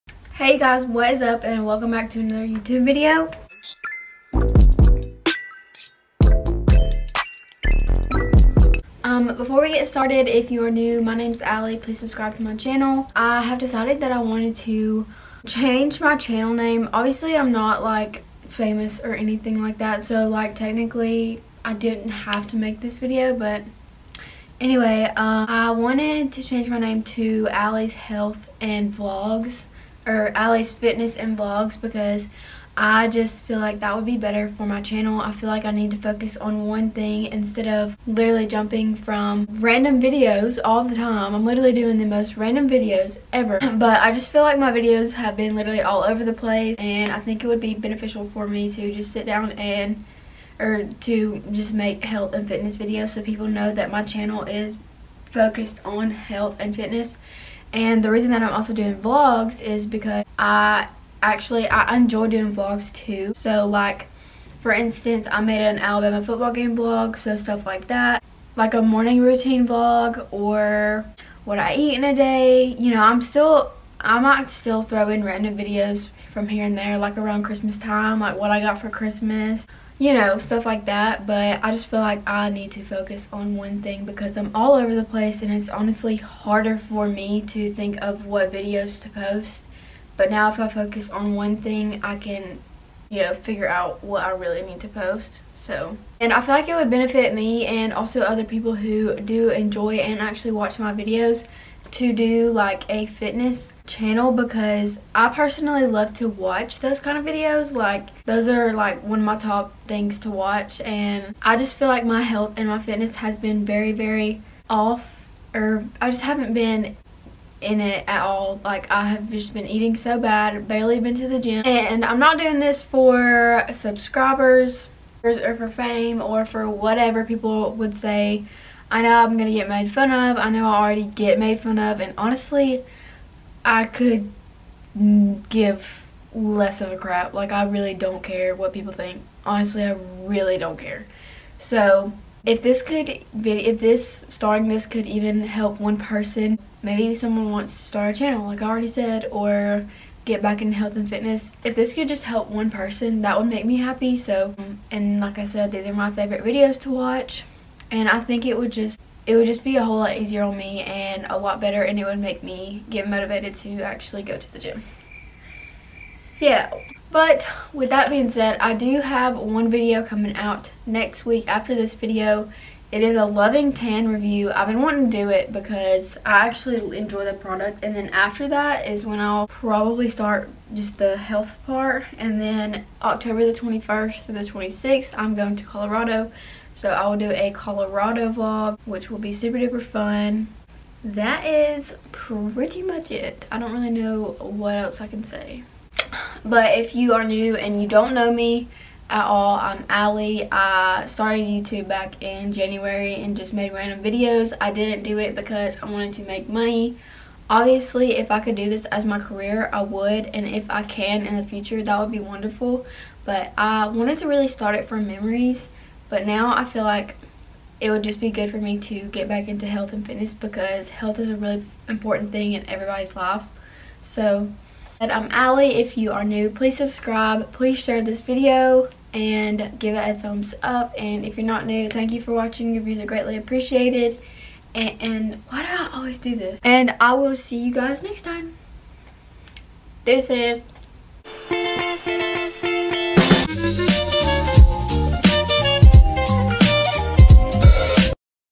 talking about why i changed my channel name...
& if you haven’t watched my most recent video of me goin to an Alabama game, don’t forget to go watch ! 🤍 love you guys